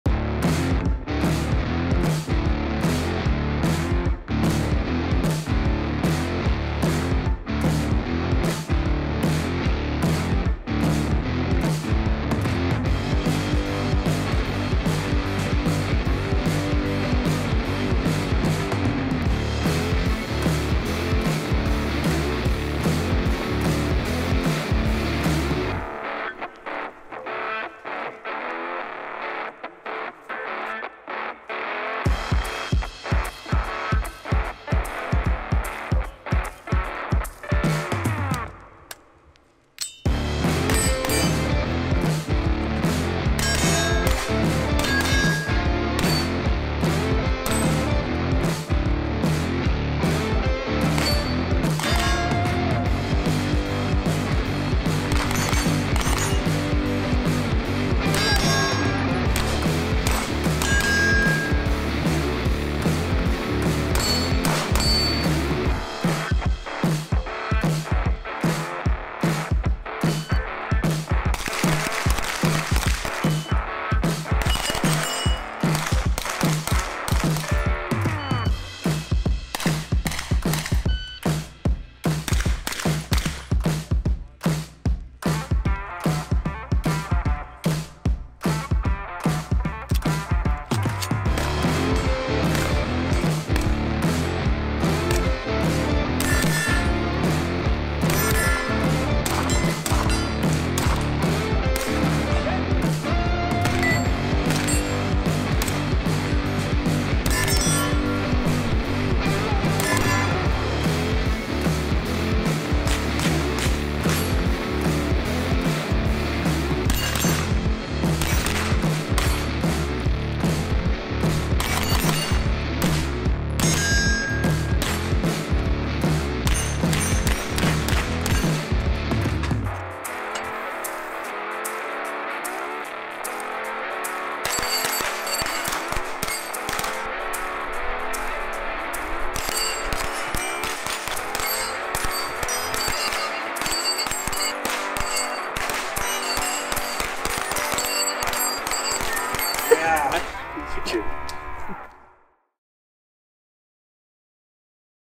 Competition PCC with JP Rifles
My buddy Max Leograndis and I put on an event here at F5 Range focused on helping you dial in your competition skills with the Pistol Caliber Carbine. PCCs are great training platforms, fun to race and this event is one we love to host at F5.